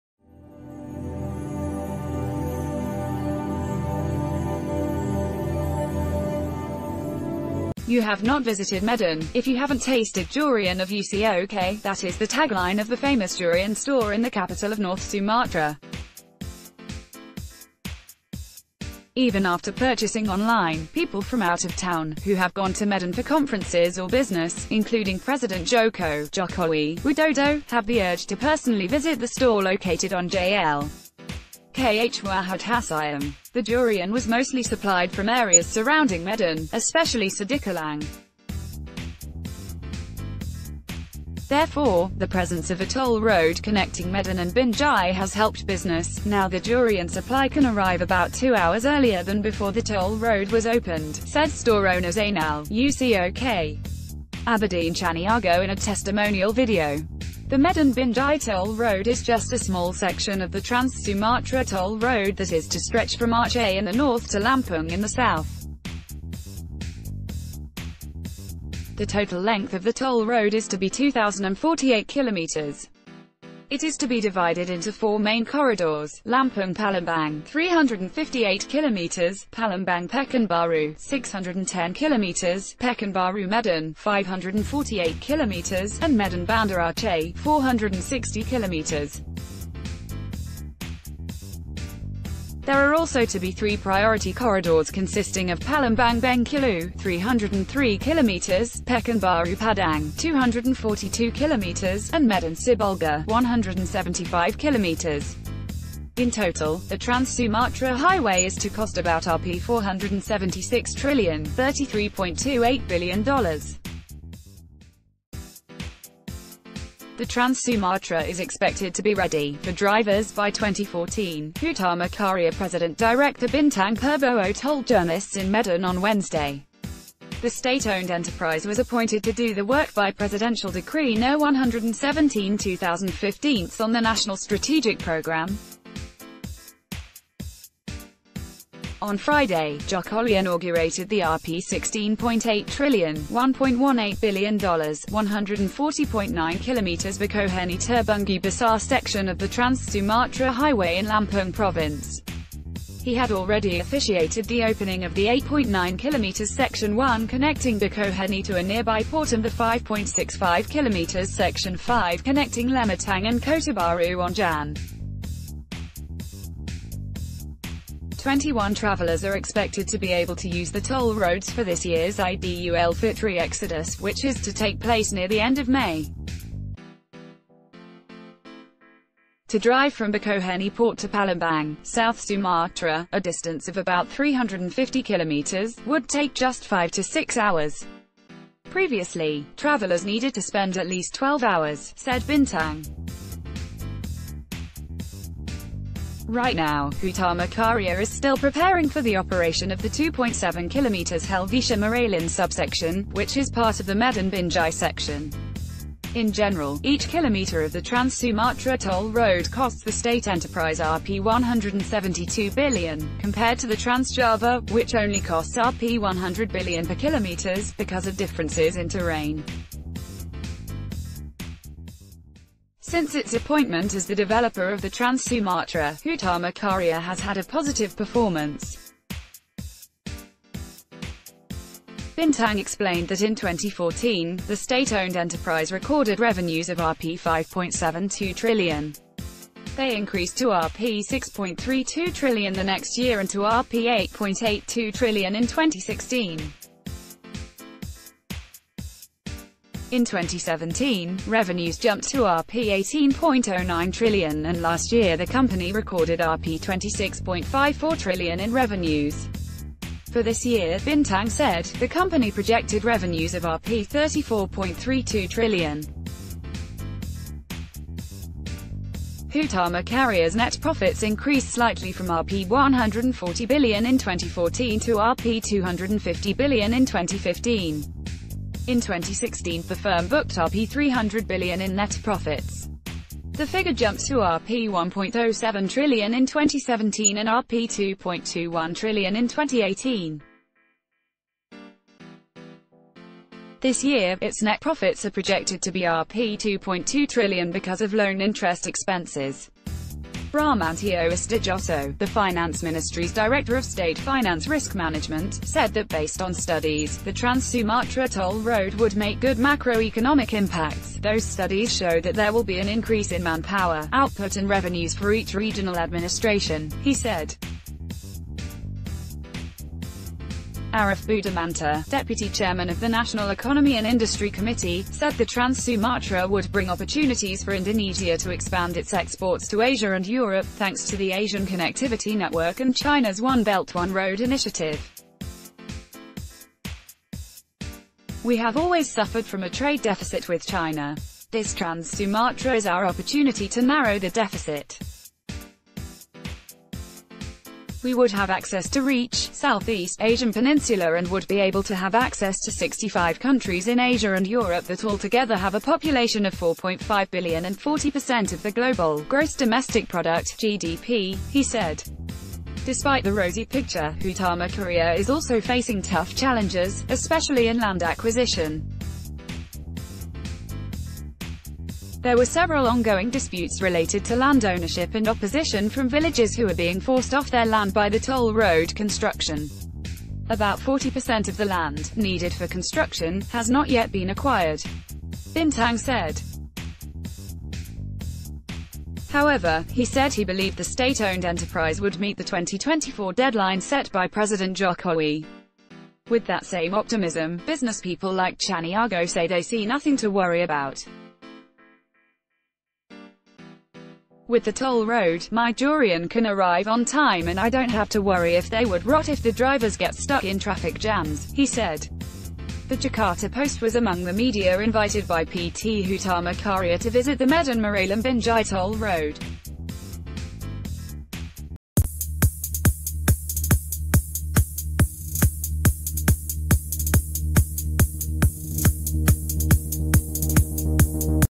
Tasting trans-Sumatra connectivity
“You have not visited Medan, if you haven’t tasted durian of Ucok” that is the tagline of the famous...